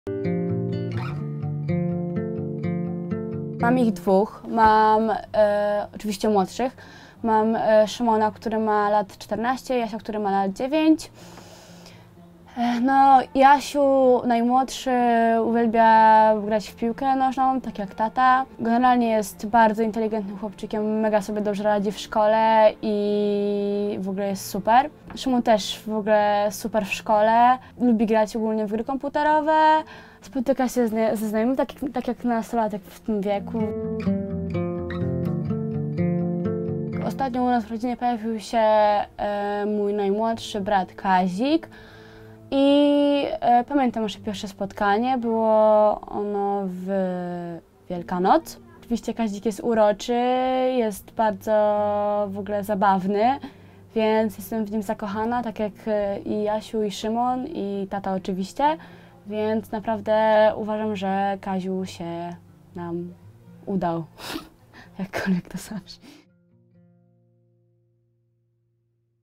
Jak Oliwia Bieniuk poznała przyrodniego brata? „Kaziu się nam udał!”
Szymon, Jan, a od niedawna także Kaziemierz - Oliwia Bieniuk ma spore rodzeństwo. W wywiadzie z VIVĄ.pl opowiedziała więcej o swoich braciach. W najmłodszym jest niemal zakochana! | Więcej